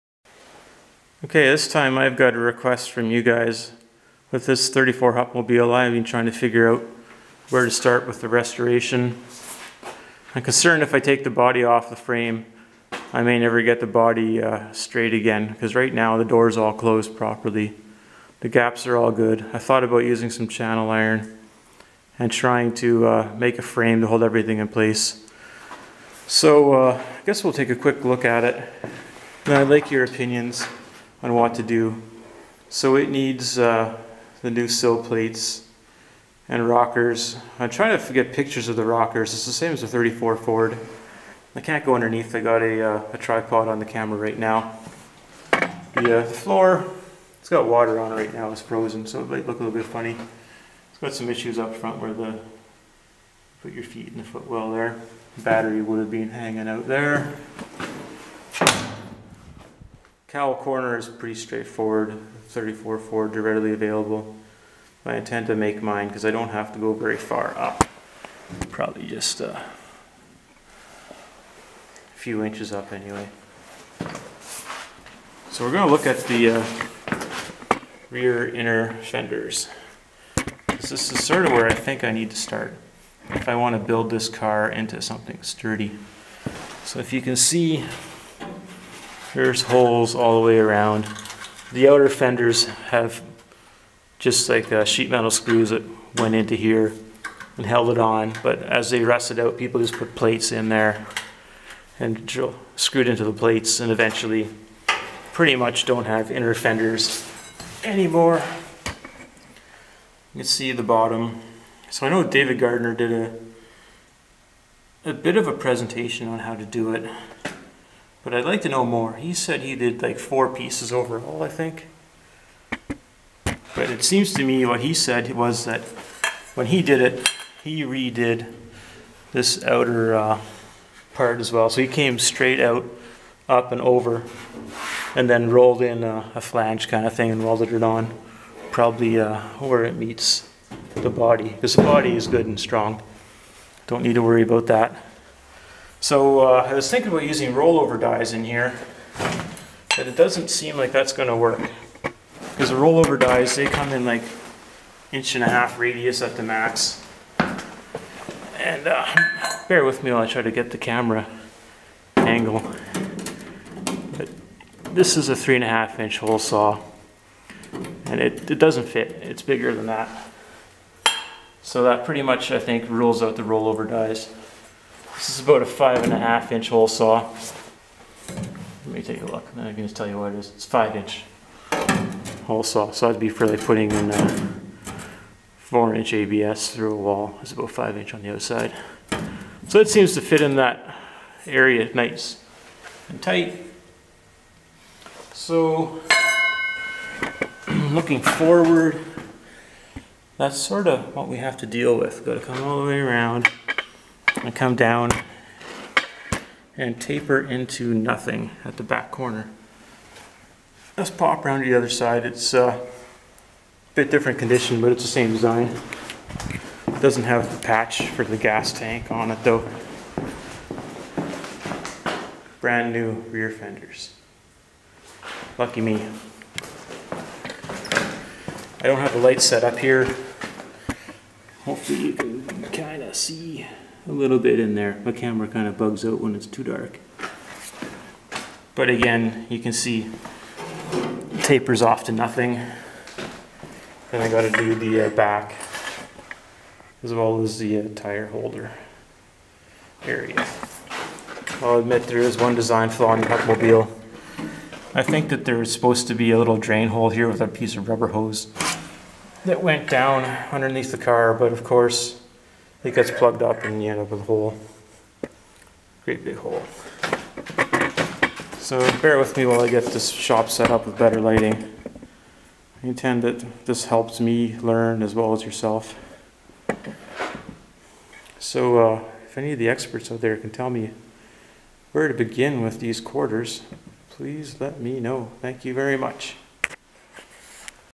1934 Hupmobile 417W - Where to Start?
This car came with a Ford 289 when I got it so please don't shun me for "rodding it".  Please provide insight on how I should begin the process of fixing the body.